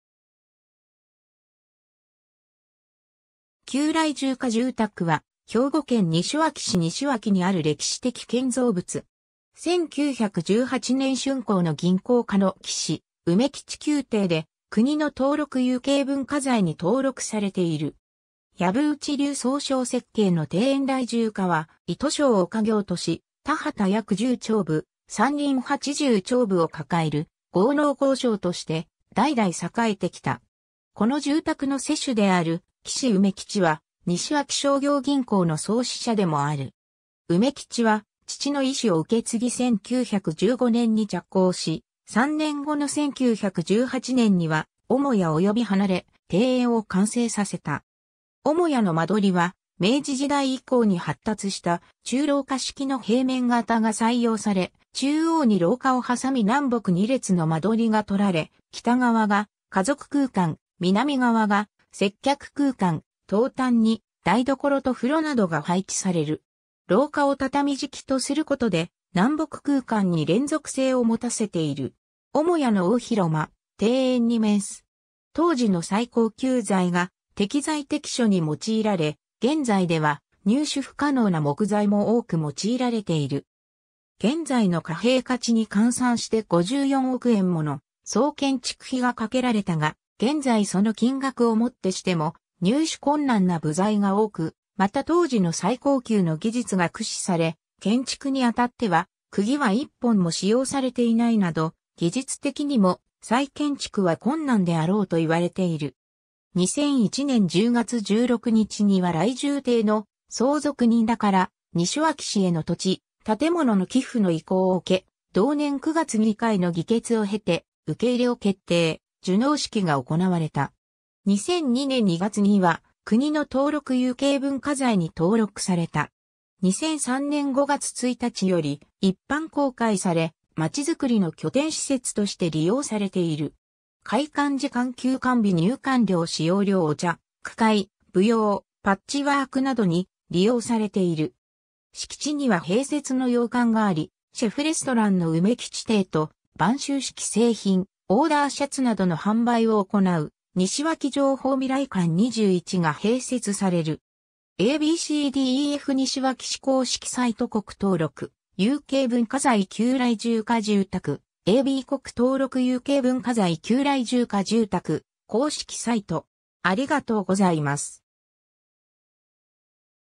旧来住家住宅
旧来住家住宅（きゅうきしけじゅうたく）は、兵庫県西脇市西脇にある歴史的建造物。
1918年（大正7年）竣工の銀行家の来住梅吉旧邸で、国の登録有形文化財に登録されている。
 藪内流宗匠設計の庭園 来住家は糸商を家業とし、田畑約10町歩、山林80町歩を抱える豪農豪商として、代々栄えてきた。
この住宅の施主である来住梅吉は西脇商業銀行（後に神戸銀行へ合併）の創始者でもある。
梅吉は父の意思を受け継ぎ1915年（大正4年）に着工し、3年後の1918年（大正7年）には母屋および離れ、庭園を完成させた。
 母屋の間取りは明治時代以降に発達した中廊下式の平面形が採用され、中央に廊下をはさみ南北2列の間取りがとられ、北側が家族空間、南側が接客空間、東端に台所と風呂などが配置される。
廊下を畳敷きとすることで南北空間に連続性を持たせている。
 母屋の大広間、庭園に面す。
 当時の最高級材が適材適所に用いられ、現在では入手不可能な木材も多く用いられている。
現在の貨幣価値に換算して54億円もの総建築費がかけられたが、現在その金額をもってしても入手困難な部材が多く、また当時の最高級の技術が駆使され、建築に当たっては釘は1本も使用されていないなど、技術的にも再建築は困難であろうといわれている。
 2001年（平成13年）10月16日には来住邸の相続人らから、西脇市への土地・建物の寄付の意向を受け、同年9月議会の議決を経て受入を決定、受納式が行われた。
2002年（平成14年）2月には国の登録有形文化財に登録された。
2003年（平成15年）5月1日より一般公開され、まちづくりの拠点施設として利用されている。
 開館時間 休館日 入館料 使用料 ※お茶、句会、舞踊、パッチワークなどに、利用されている。
 敷地には併設の洋館がありシェフレストランの梅吉亭と、播州織製品、オーダーシャツなどの販売を行う西脇情報未来館21が併設される。